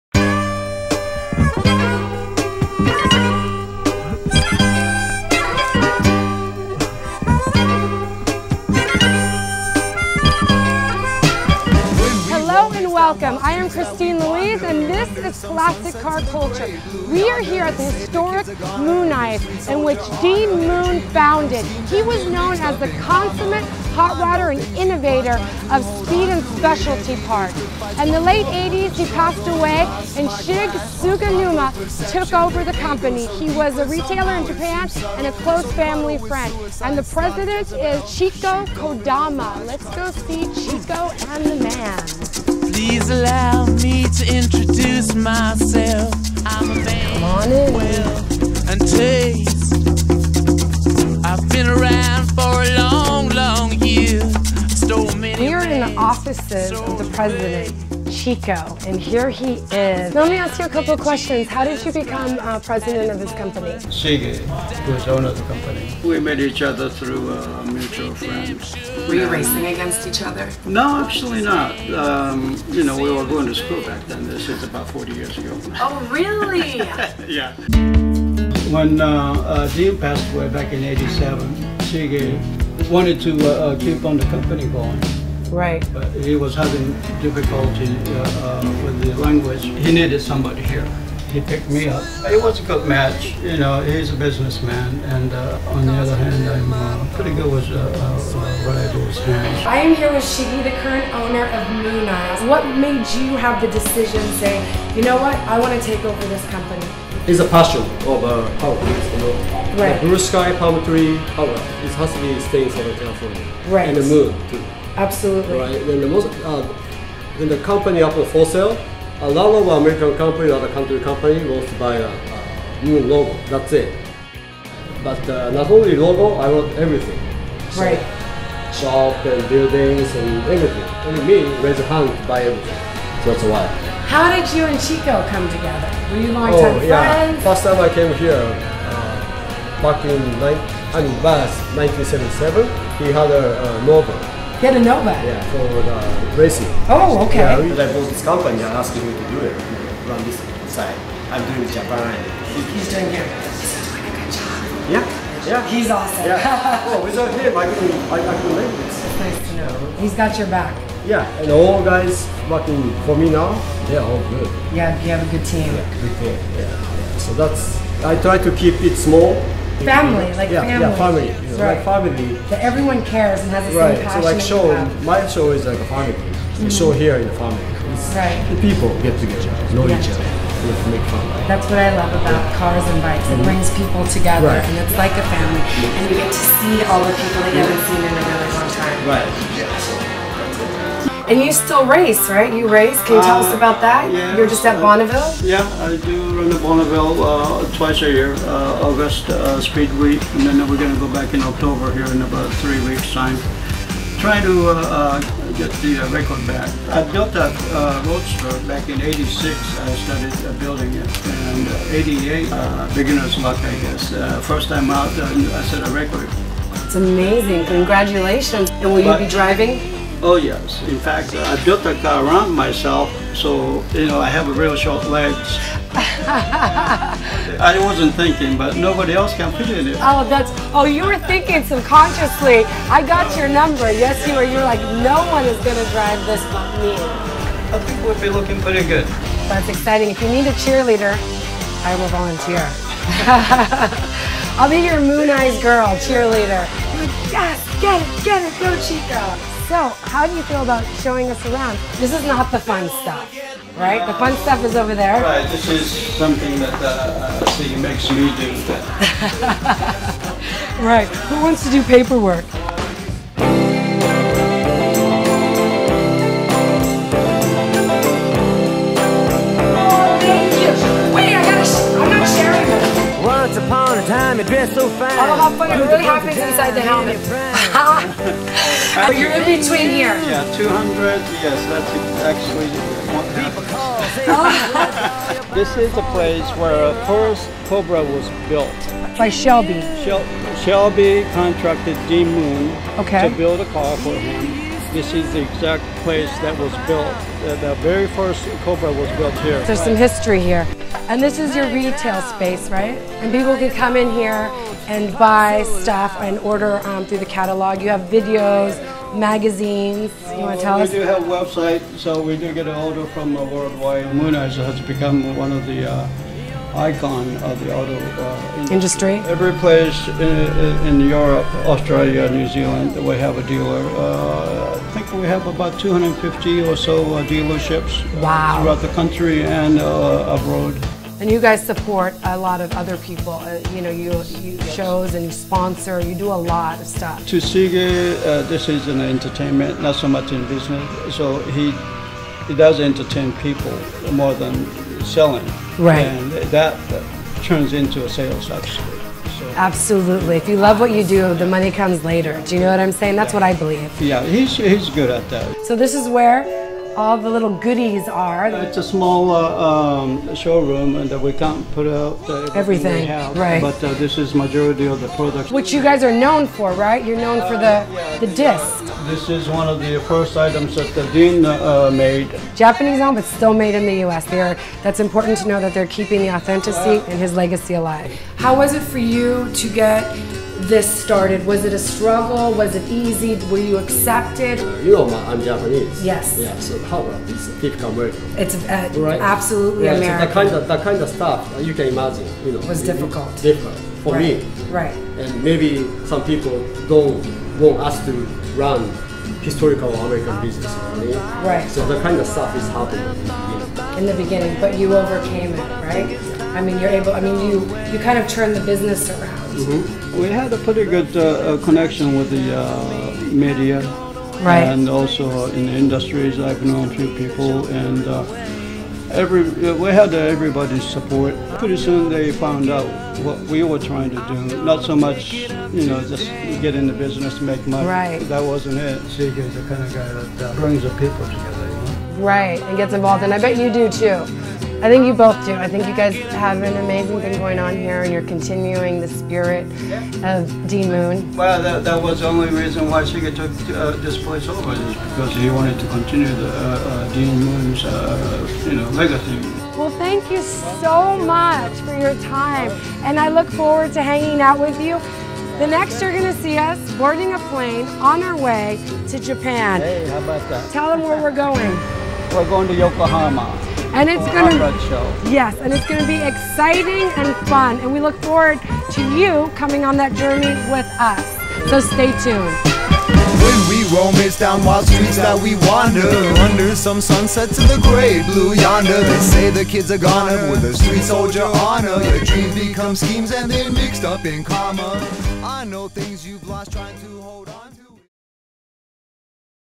MOONEYES Interviewed by Classic Car Culture
Classic Car Culture with Kristine Louise stopped by MOONEYES shop in Santa Fe Springs, CA to interview Chico and Shige. They sent us this video clip. Thank you!